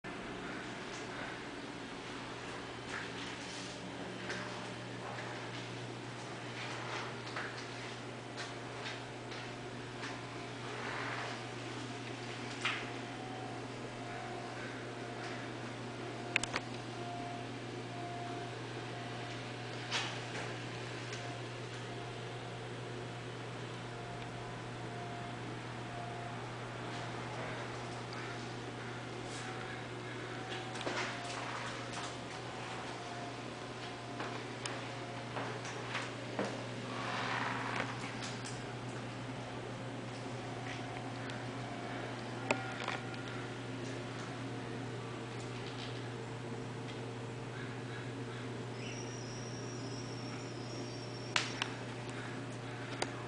bootvideo 271 - Bathroom Stall
Checking out some dude's brown lizard cowboy boots.